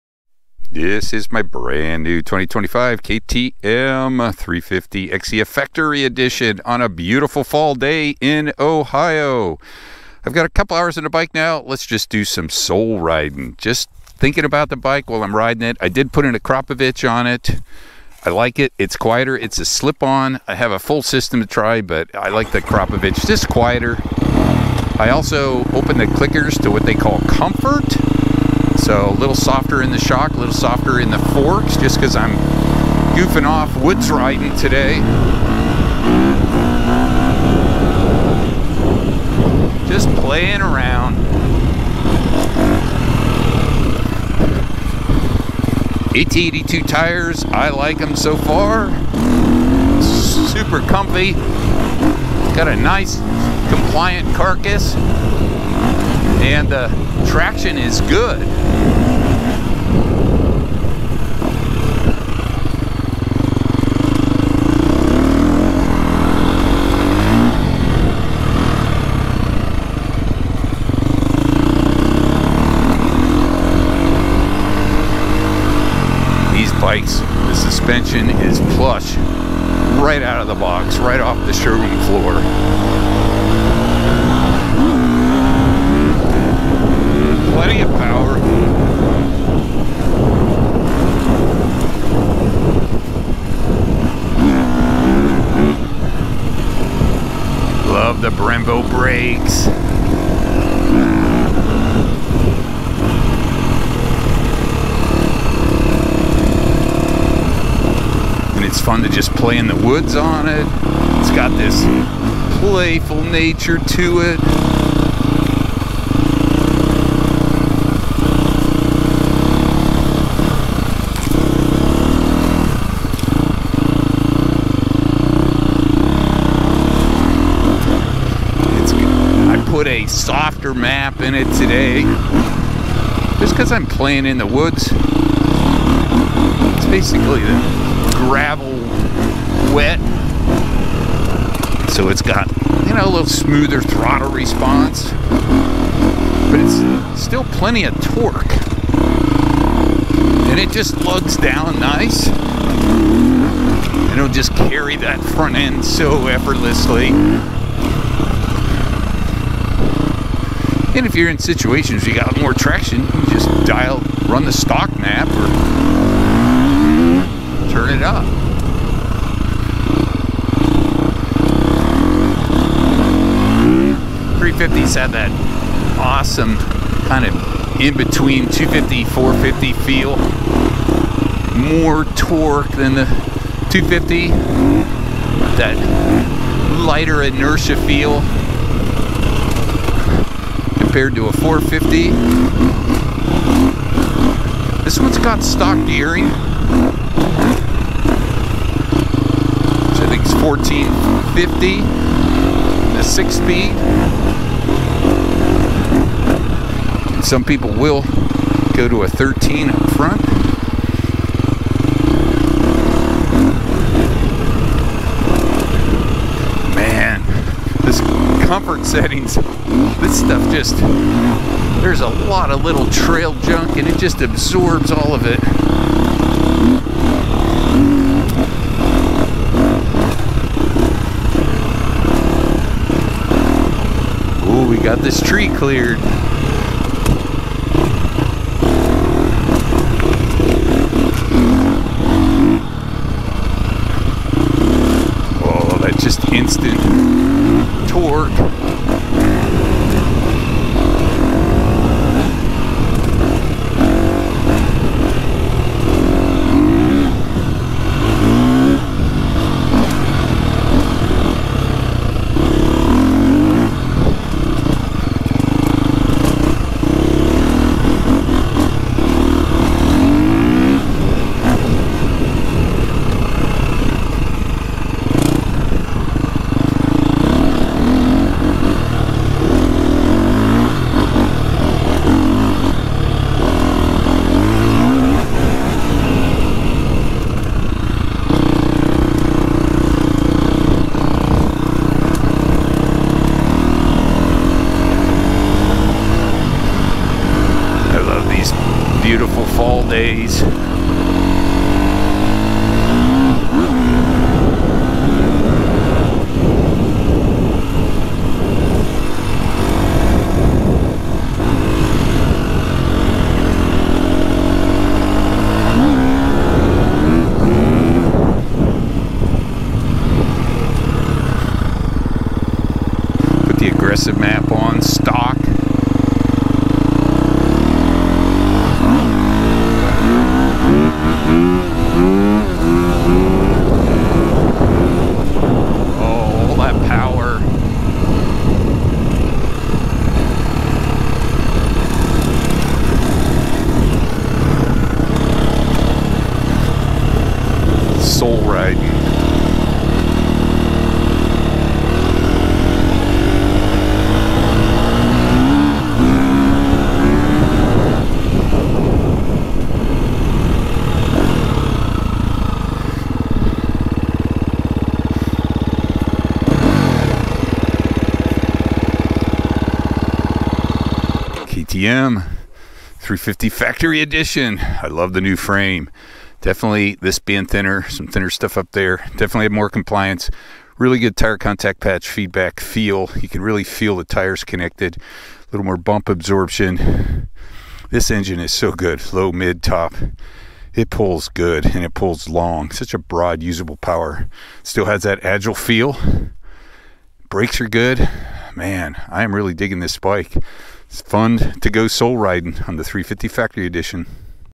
2025 KTM 350XCF Factory Edition Soul Riding!
Soul riding on a beautiful fall day in Ohio on my 2025 KTM 350XCF Factory Edition (big thanks to my good buddy Wally at Wheelsports, Columbus, Ohio). This is bike is FUN! Factory Editions have updated parts that improve performance and also look cool. Austrian 350's are my favorite combination of Power/Weight/Handling in a 4-stroke dirt bike!

350's are such a great combination of Power/Weight/Handling and this latest 350 engine in the updated 2025 KTM chassis with new WP spring forks does not disappoint!  The latest 350 engine has stronger low to mid power compared to the previous generation engine and still pulls forever on top and both of the stock EFI maps run clean and crisp.  The Factory Edition comes with an FMF slip on muffler with a spark arrester installed.  With the Connectivity Unit Offroad it is easy to dial in the power character of the 350XCF.  For woods riding I dial back the power and throttle response using the Gravel/Wet base power map.  In the Advance settings option I minimize throttle response, minimize engine braking, and maximize Traction Control.  This combination really smoothes out the initial throttle response making the 350XCF easier to ride on tight trails.  Minimizing engine braking frees up the chassis and gives the bike a "lighter" feel in motion.  At the MX track I can use the CUO to turn up the throttle response and use the Sand/Dry base map for a little more power feel.  The CUO offers the option to change the engine power and character.

The updated stock WP spring forks are impressive!  The rear of the bike has more comfort too with the updated frame along with linkage and shock changes.  This updated 25 chassis is a great combination of cornering and stability and precision.

More rides and videos soon!